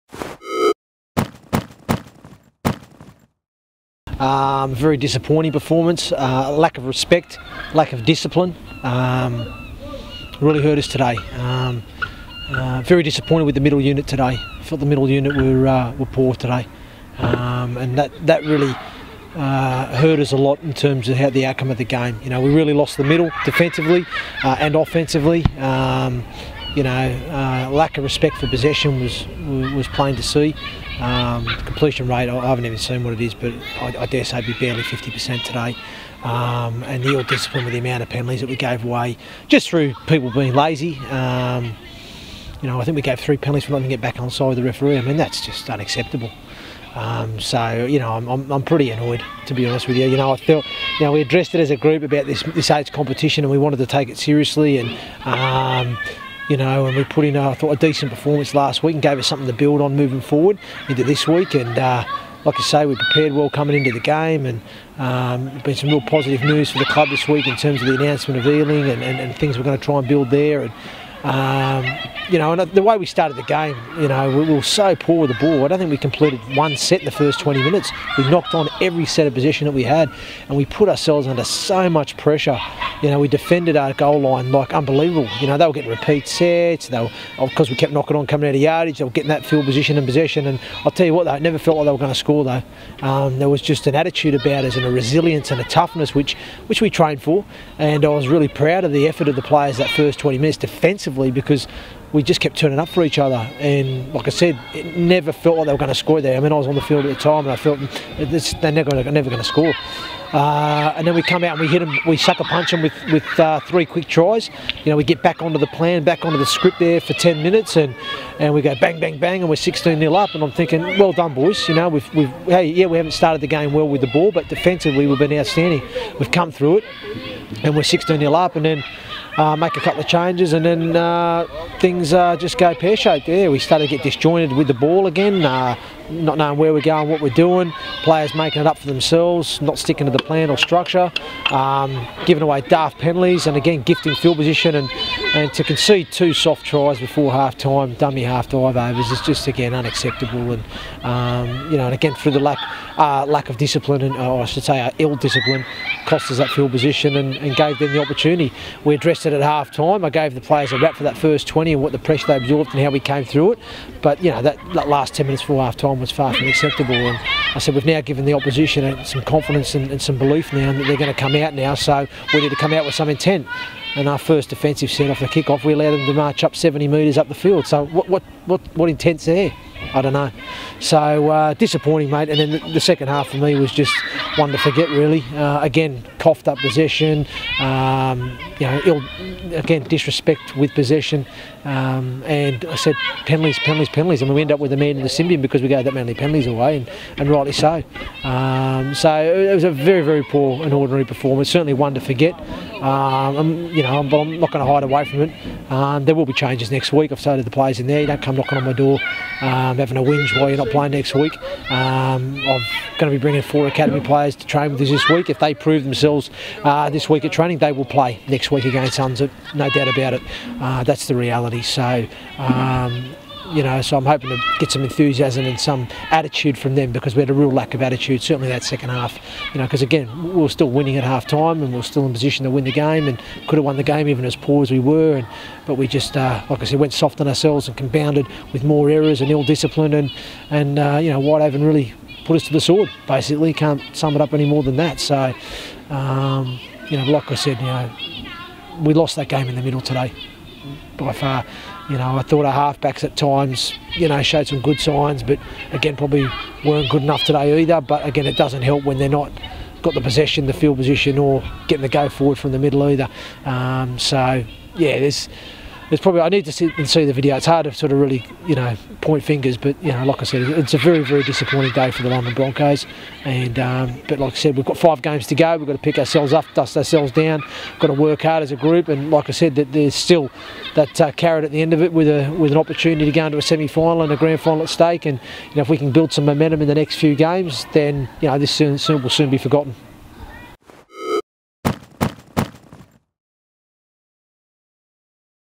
Broncos TV: Henderson post Whitehaven
We spoke to head coach Andrew Henderson after his side's defeat to Whitehaven.

Apologies for the poor focusing, it was almost pitch black when recording.